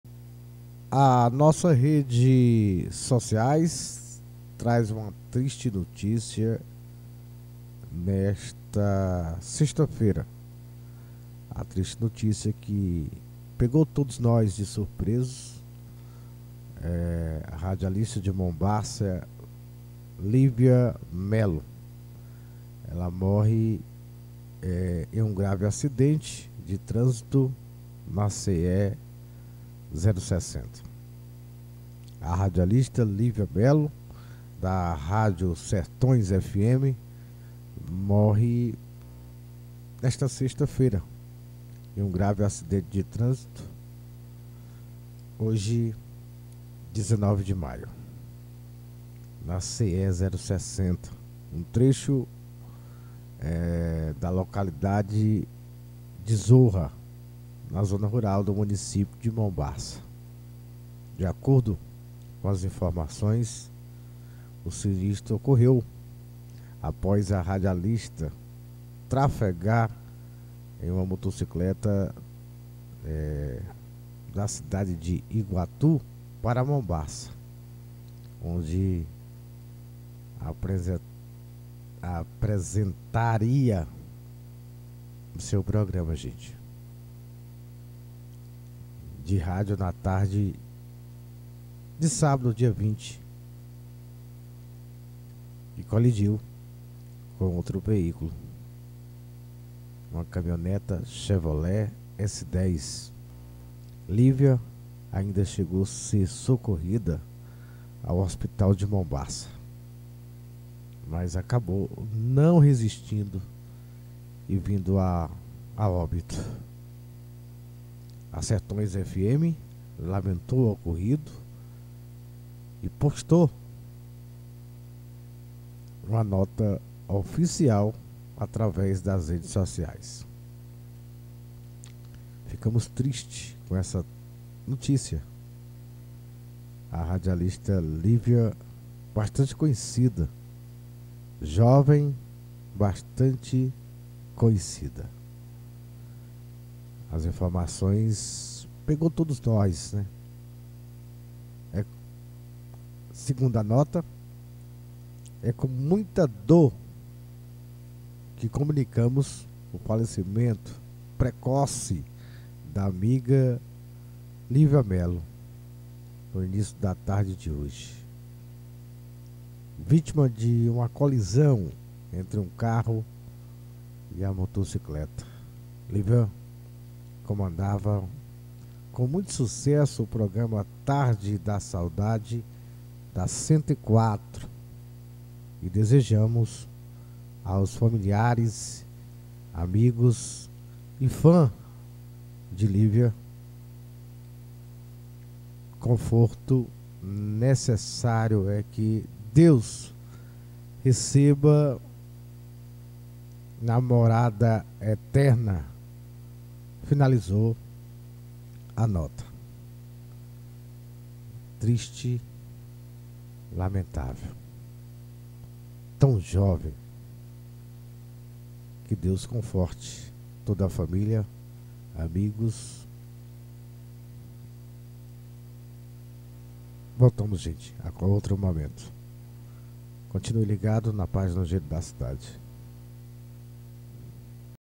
Radialista de Mombaça, Lívia Melo, morre em grave acidente de trânsito na CE-060
A radialista Lívia Melo, da Rádio Sertões FM, morreu em um grave acidente de trânsito na tarde desta sexta-feira (19), na CE-060, trecho da localidade de Zorra, na zona rural do município de Mombaça.

FONTE
informações do blog do Wilrismar